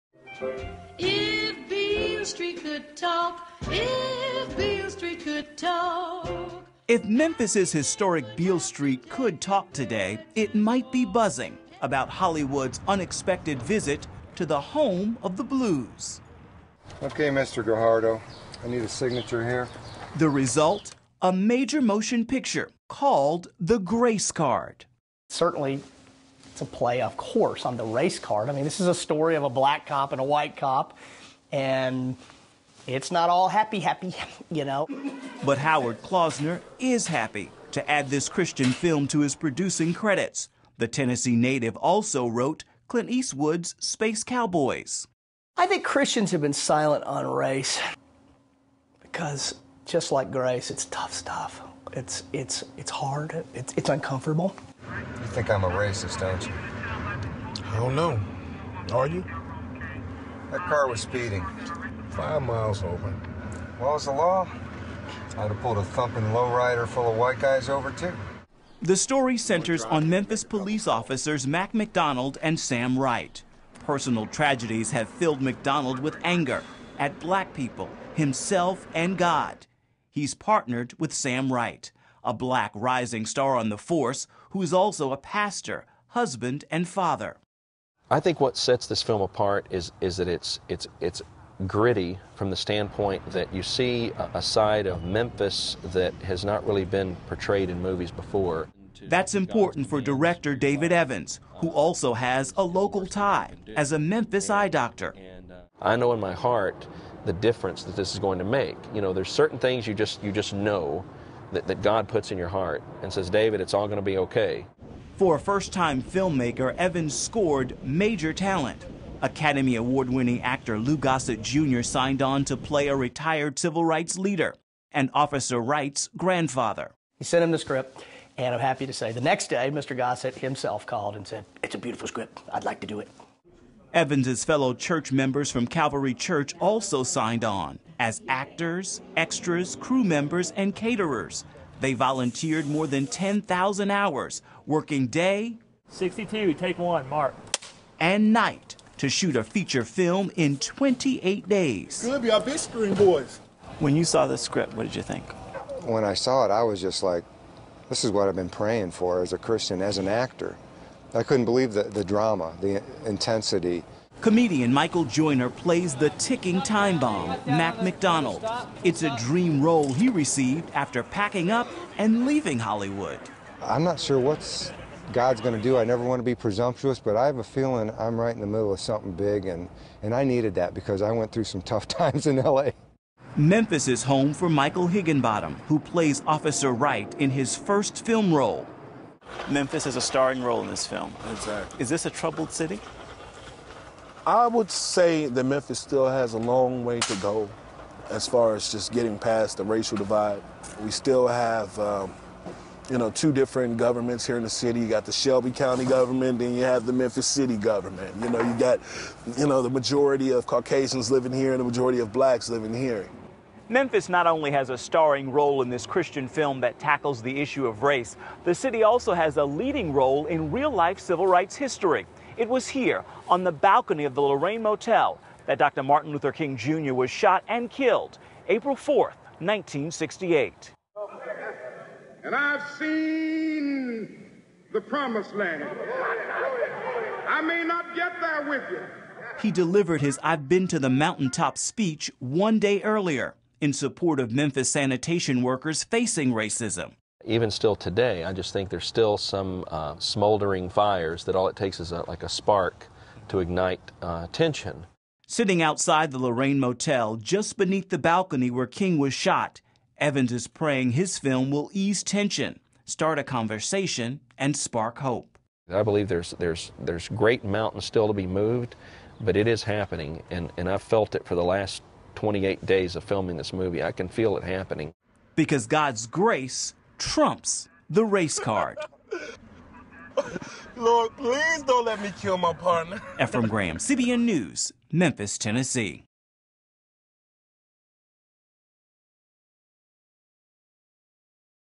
New Christian Film Brings Light to Racism, Hate - CBN.com
Producers hope the new Christian film "The Grace Card" will help bring light to racial problems that still exist today and how God's Word can help in solving them. .. The Christian Broadcasting Network CBN